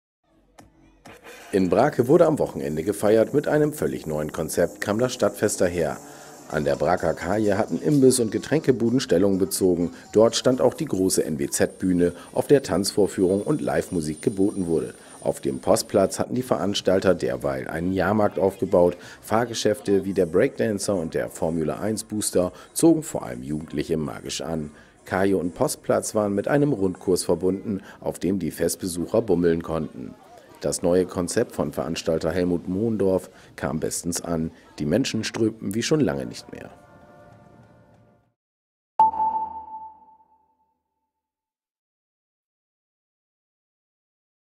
Neues Konzept belebt Stadtfest
Brake, 16. Juni 2013: Mit neuem Konzept zum Erfolg: Das Stadtfest war so gut besucht wie lange nicht mehr. Vor allem der Jahrmarkt auf dem Postplatz zog vor allem Jugendliche magisch an.